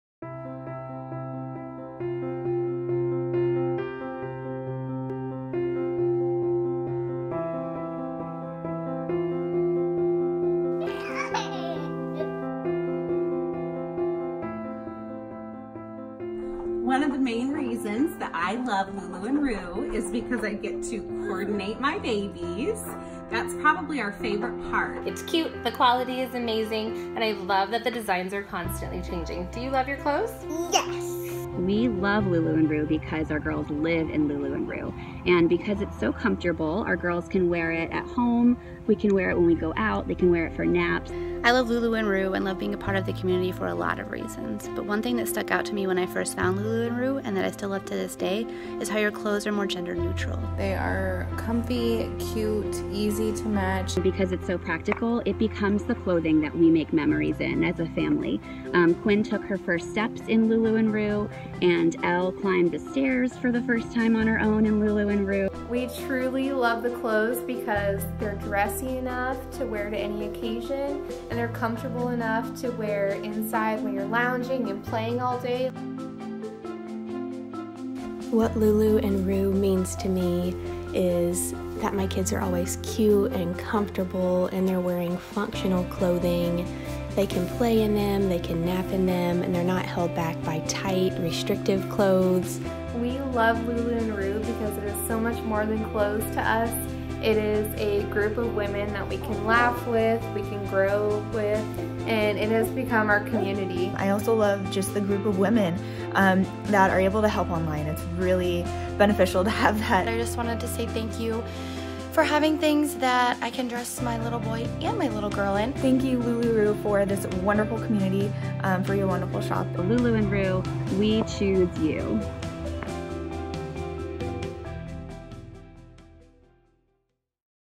Why You Love Lulu + Roo
Providing high quality and comfort for thousands of children worldwide. See what some of our customers have to say about why they love Lulu + Roo for their kids.